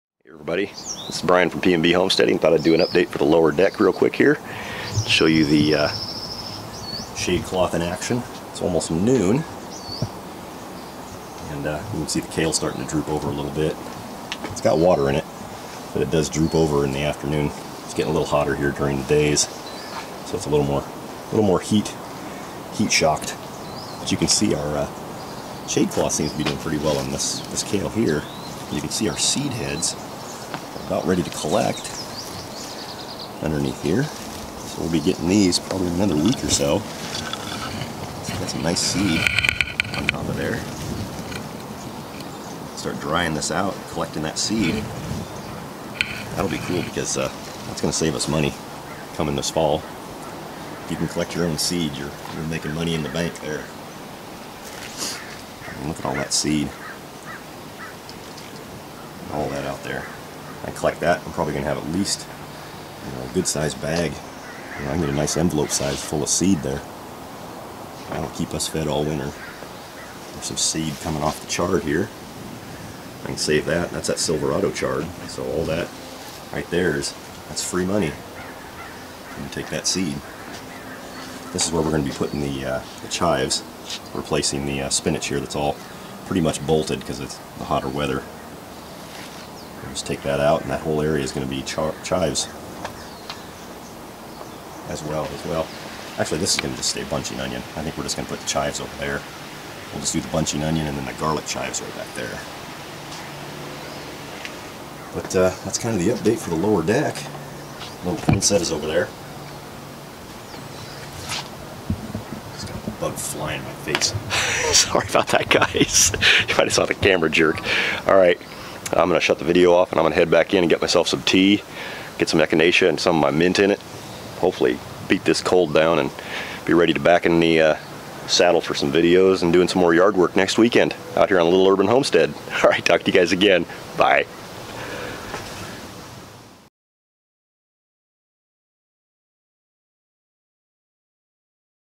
Lower deck raised garden beds update week 25
Hi Everyone, here's a quick update for the lower deck garden boxes. Enjoy the video update! :-)